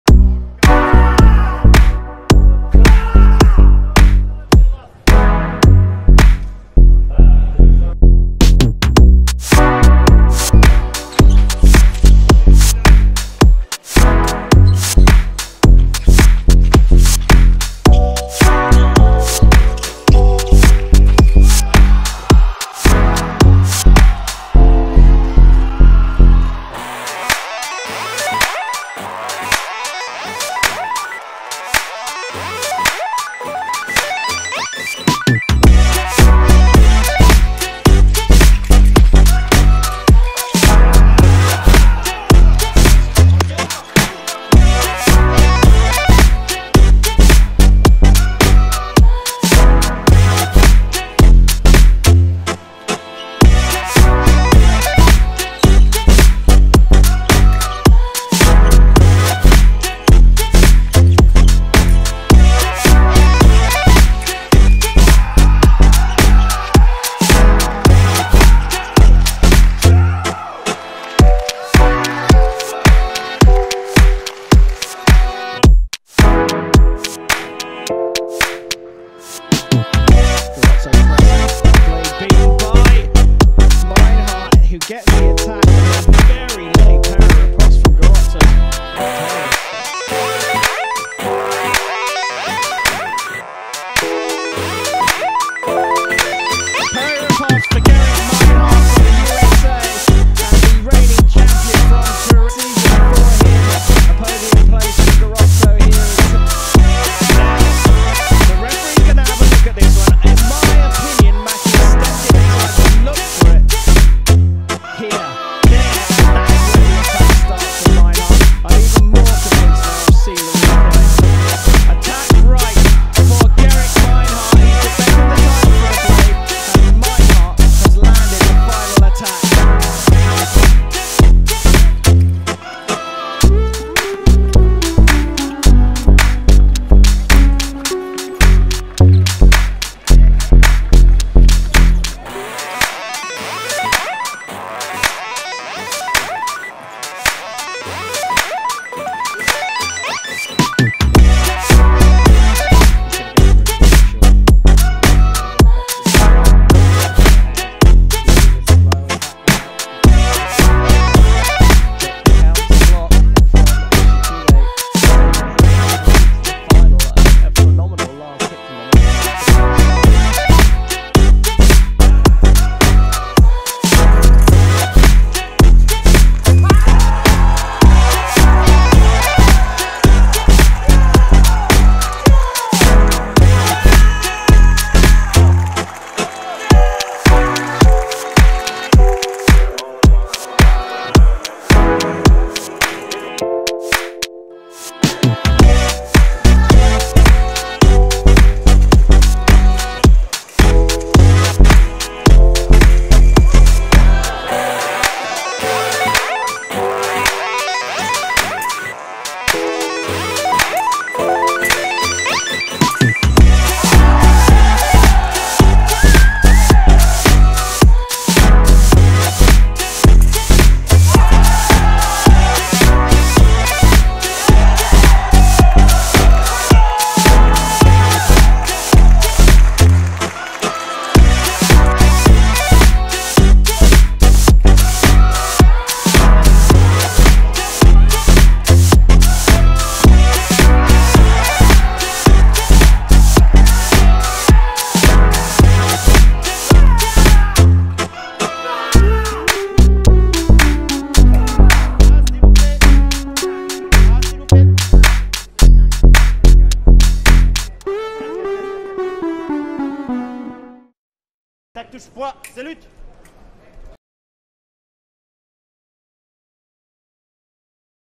Which fencer's ceremony is the coolest?
Fencing 14-14 compilation.
Who is your favorite fencer?